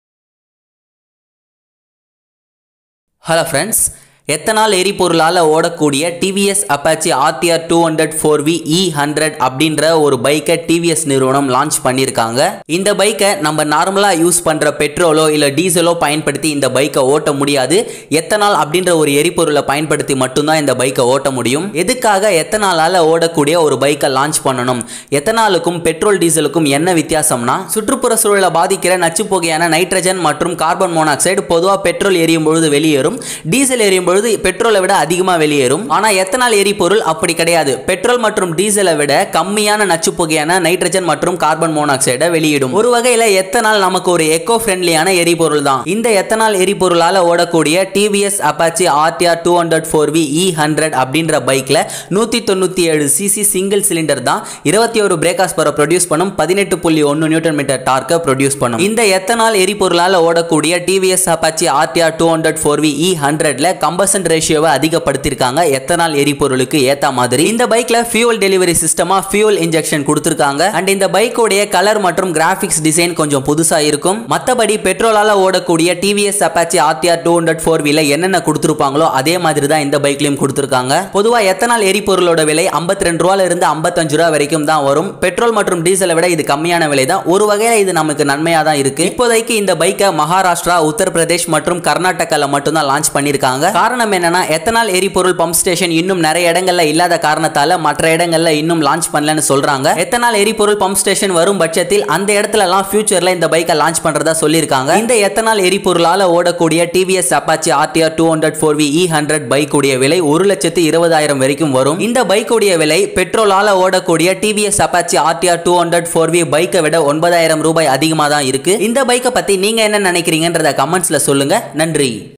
TVS Apache RTR 200 4v Fi E100 ethanol powered bike launched | தமிழில் | Mech Tamil Nahom
Ethanol powered TVS Apache RTR 200 4V Fi E100 bike introduced by TVS and it's just launched an ethanol-powered variant of the Apache RTR 200 4V and is calling it the Apache RTR 200 Fi E100 and this video will be explained in Tamil language and thank you for watching this video